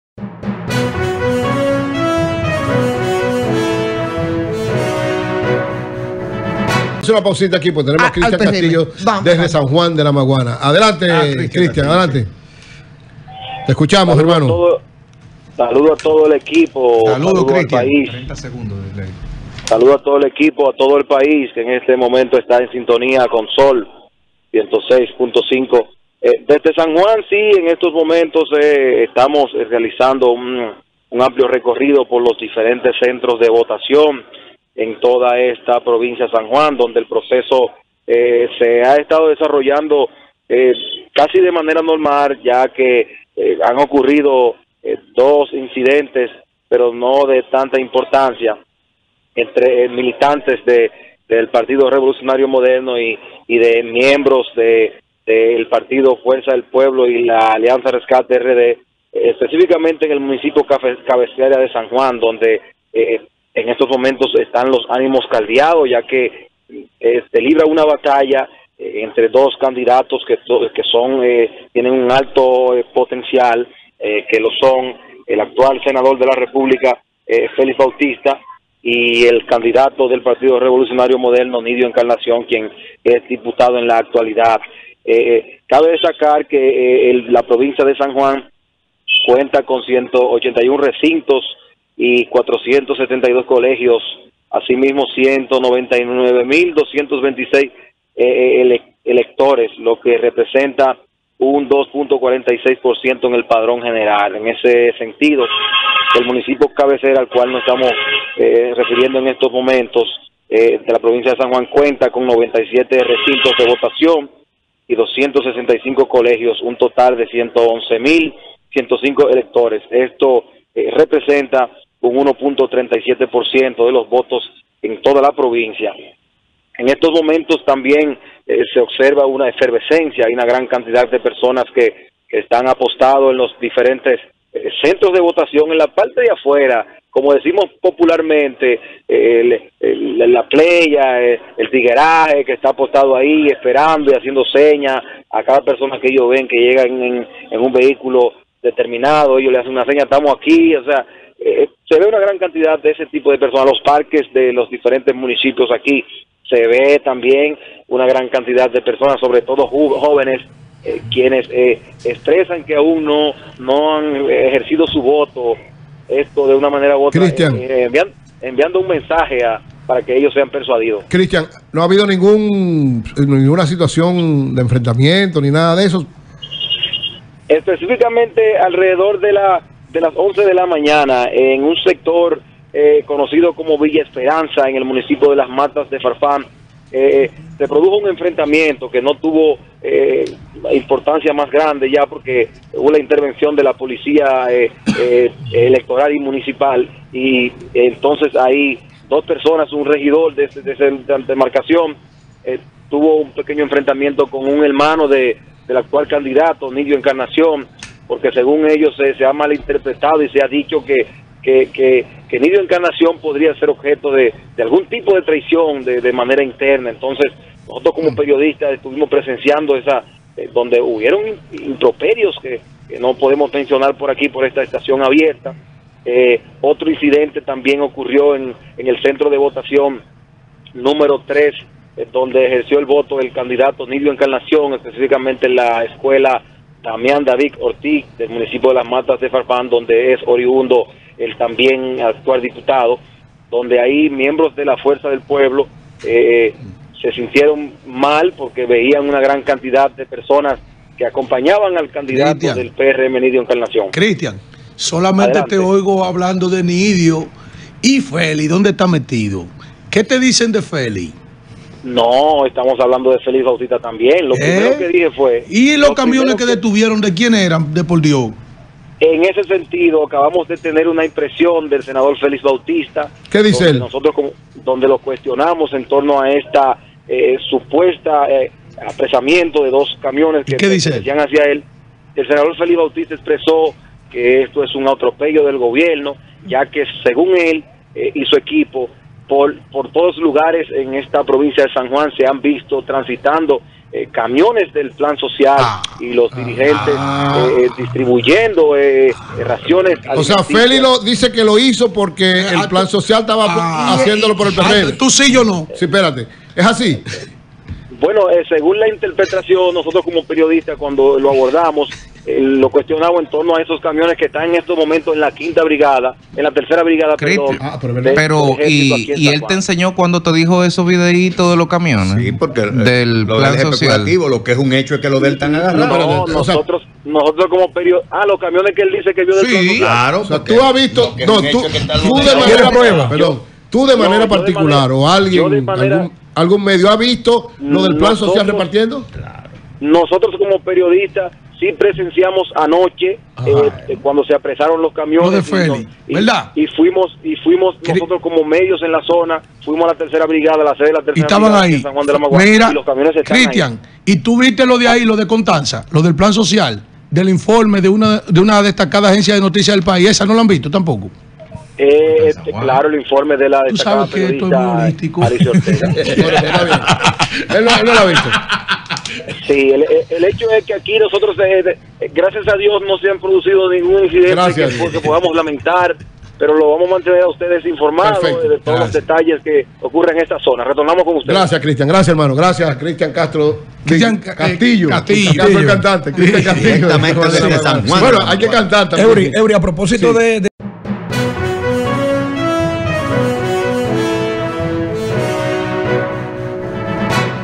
CRISTIAN CASTILLO REPORTA DESDE SAN JUAN PROCESO DE LAS ELECCIONES 2024
Accede a todo nuestro contenido en Instagram, Facebook y Twitter como @rccmediard
¡La red de medios más grande e importante de la República Dominicana!